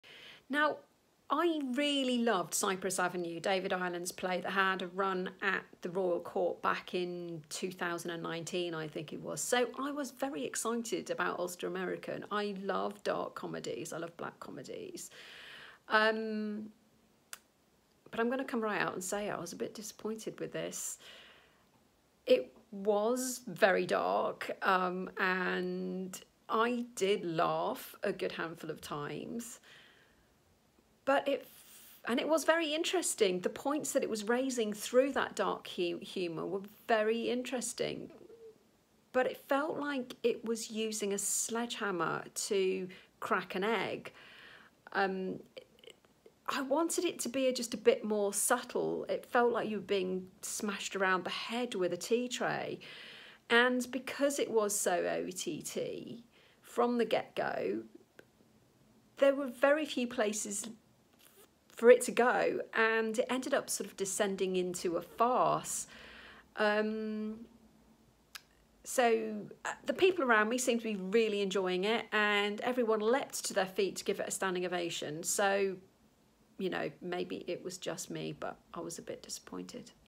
Review: Ulster American, Riverside Studios
An English director, famous American actor and Northern Irish playwright meet the day before rehearsals start on a dark, violent new play set in Northern Ireland.

Here are a few thoughts, and I’ll put more on the blog.

Have you seen it?

Ulster American, Riverside Studios
Written by David Ireland
Directed by Jeremy Herrin
Starring Andy Serkis, Woody Harrelson and Louisa Harland